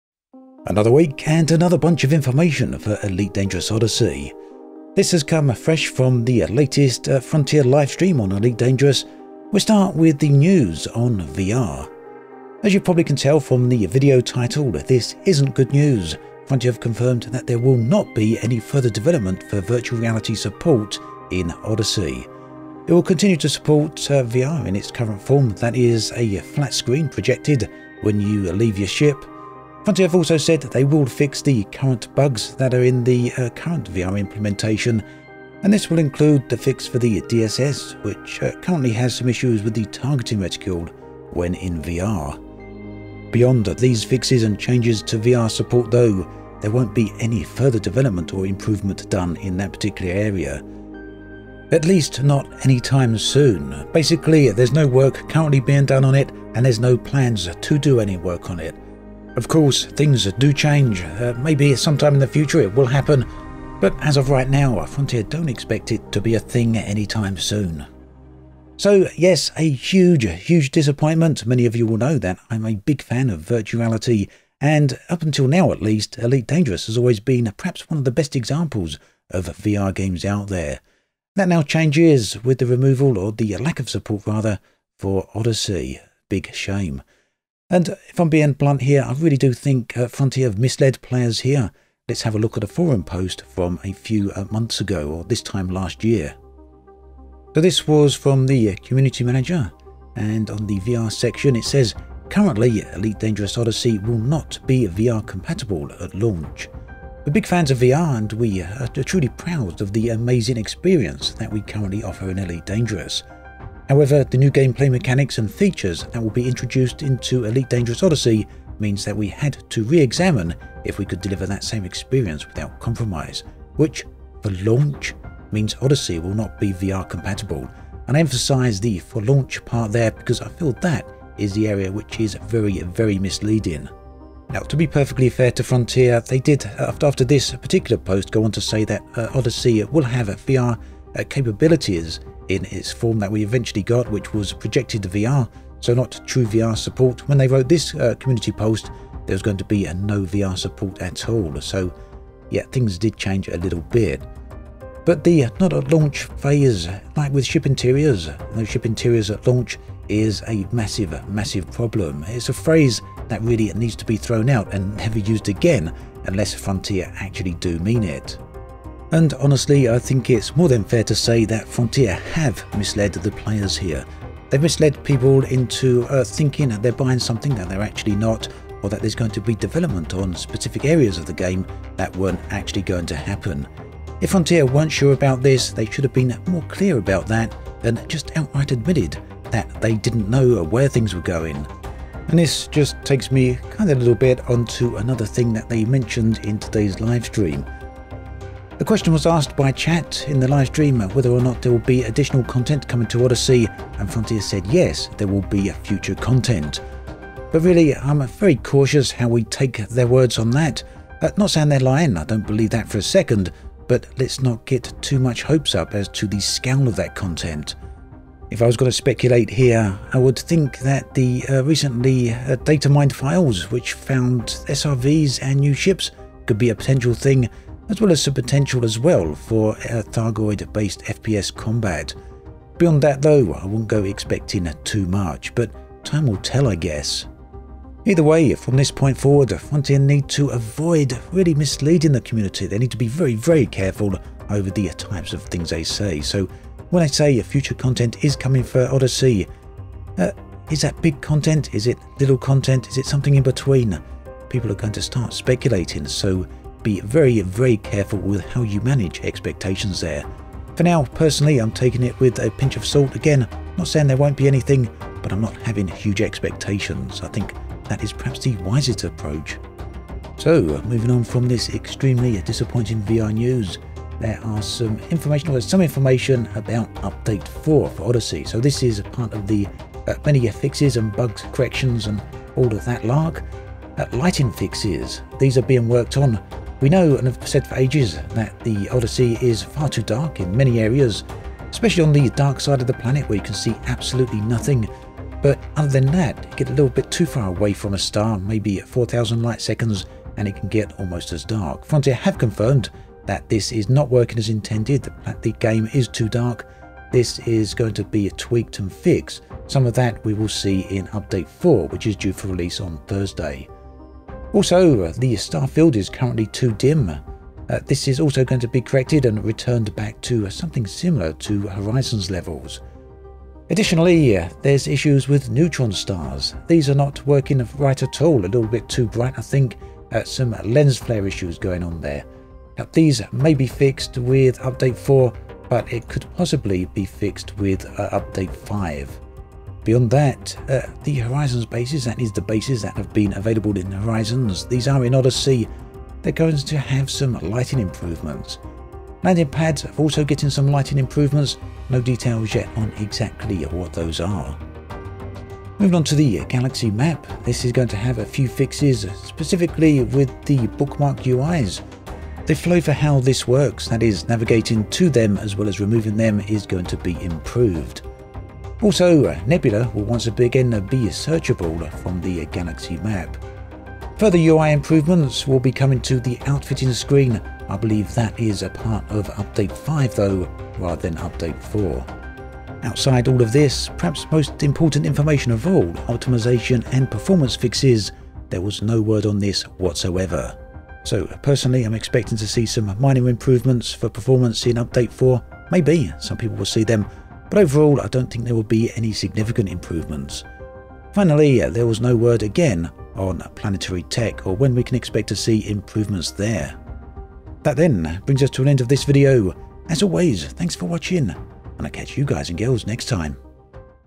Elite Dangerous - No Further VR Development, Lighting Fixes and Improvements, Bug Fixes
The latest info for Elite Dangerous Odyssey confirms that VR will not be getting further development.  Lighting fixes are coming for Update 4, along with some UI improvements.  Update 5 meanwhile will bring some changes to the Outfitting UI screen.  Finally, Frontier will give an update on planet tech and performance at the end of June.

The VOD for the full Elite Dangerous Odyssey livestream can be found here: